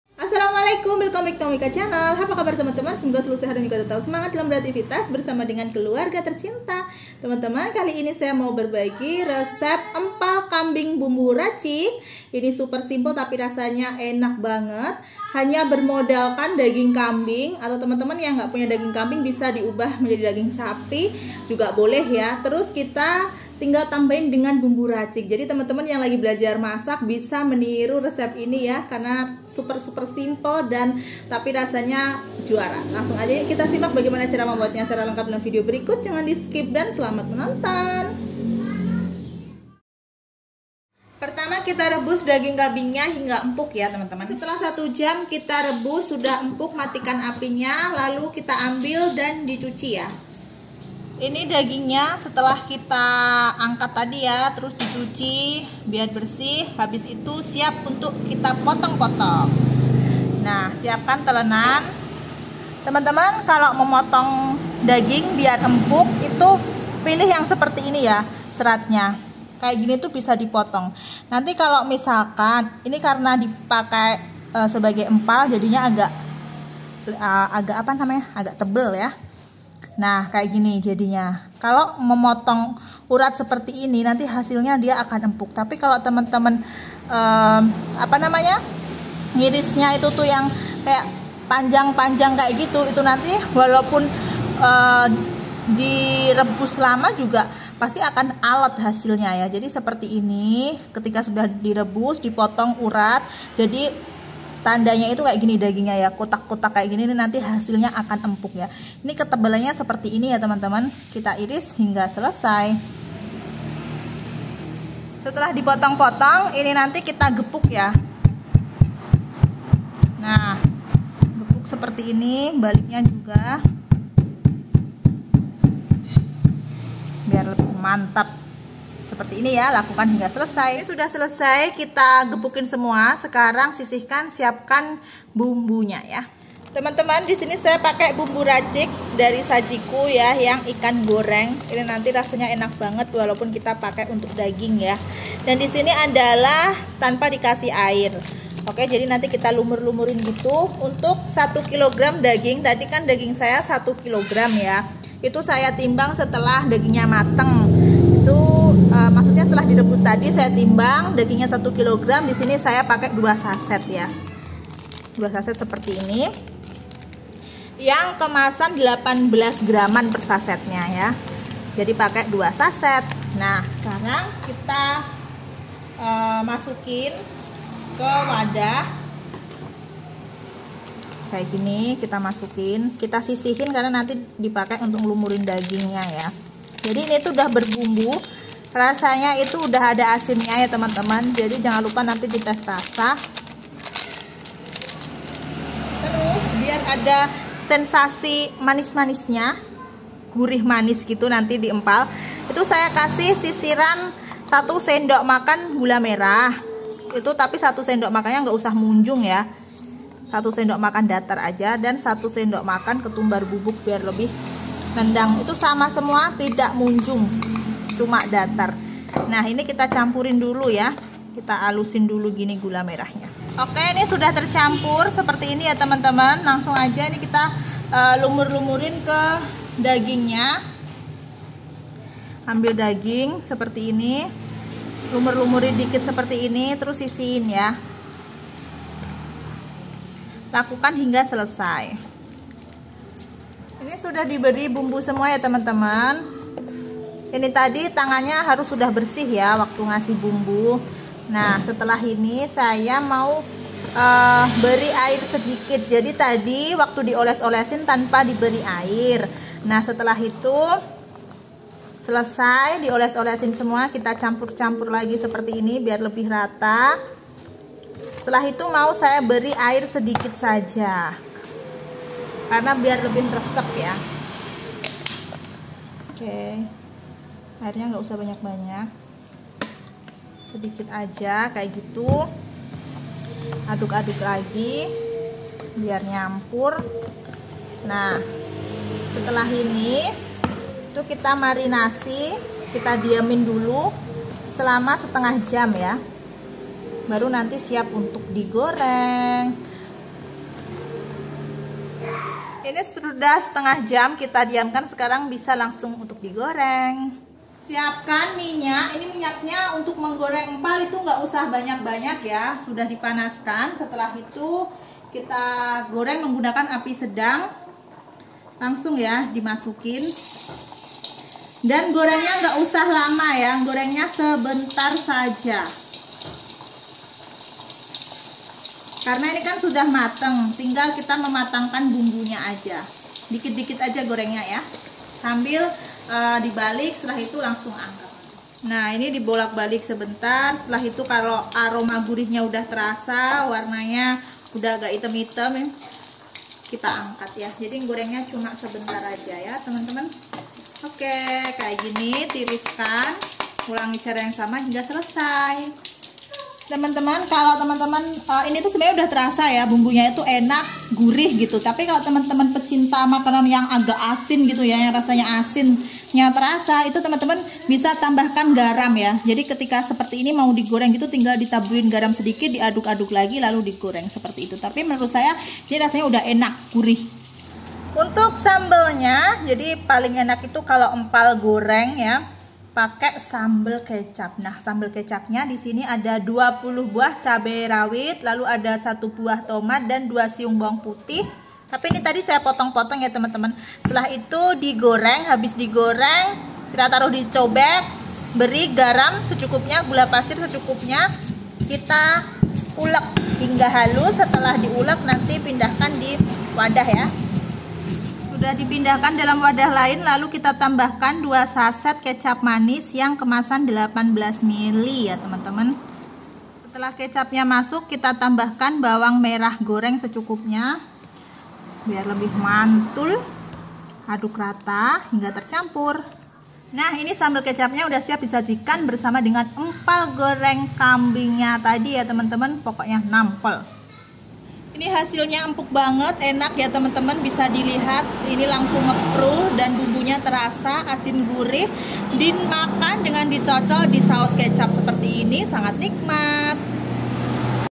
RESEP EMPAL GORENG BUMBU RACIK ENAK EMPUK GURIH
Resep Empal Goreng Bumbu Racik :
- 1 kg daging kambing/sapi
- 2 sachet bumbu instan ikan goreng sajiku kemasan 18gr
- 1 sdm datar gula merah
- 1 sdm datar ketumbar bubuk
- Sedikit air
- Sedikit garam (optional bagi pecinta makanan yg terasa asinnya)

Bahan Sambal :
- 20 buah cabai rawit
- 2 siung bawang putih
- 1 buah tomat
- Minyak goreng
- 2 sachet kecap manis
- Bawang merah goreng